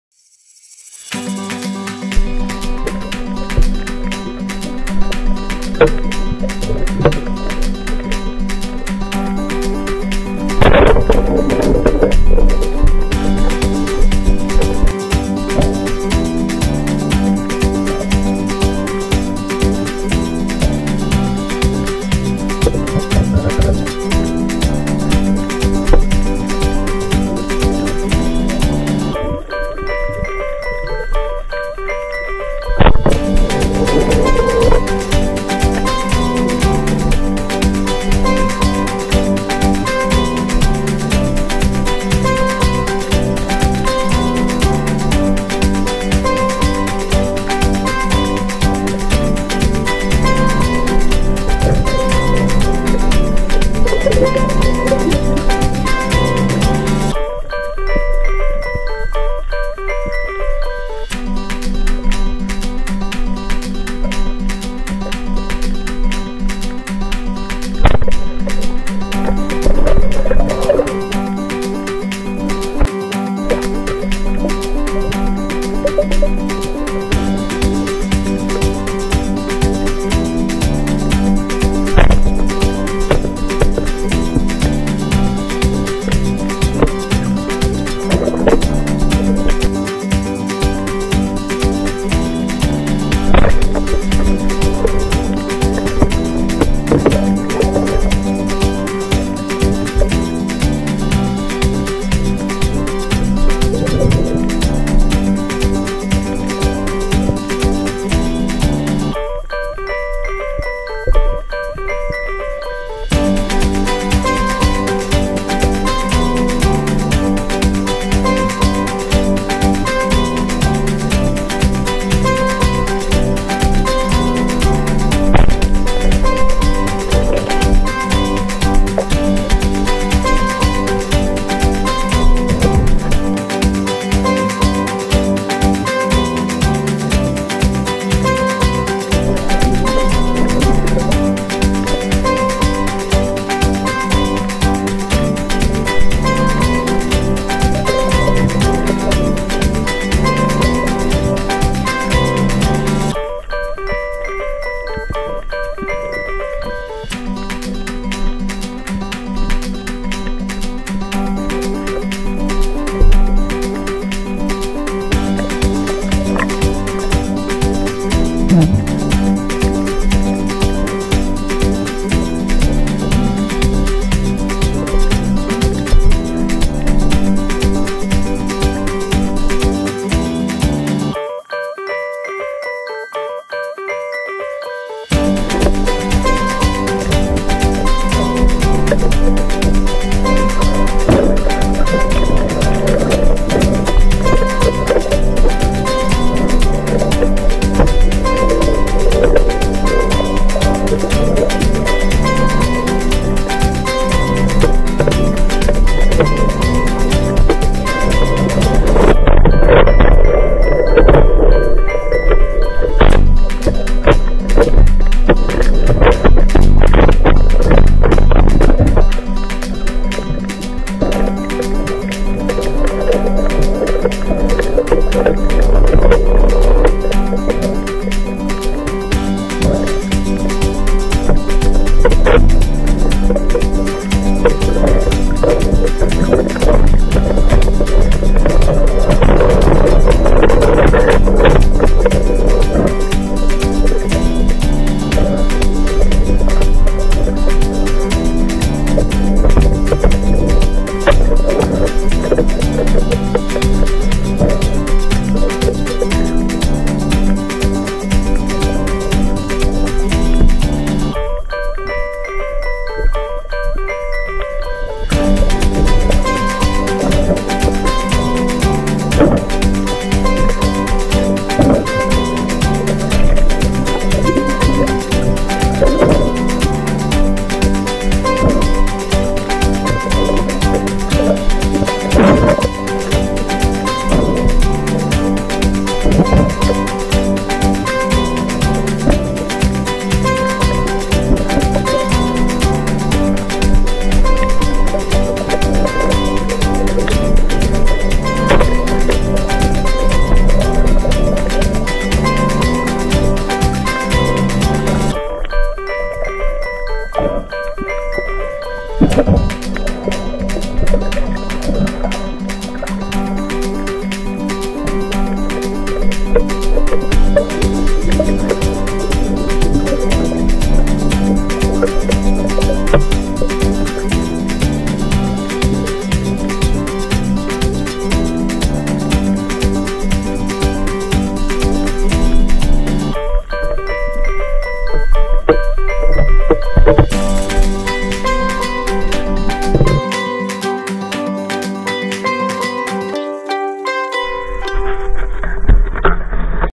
VINCY TOP DIVER-Worst day ever 100% miss (spear fishing).
Every fisherman knows that every day is fishing day, but not every day is catching day. I almost made it back home with nun but sand!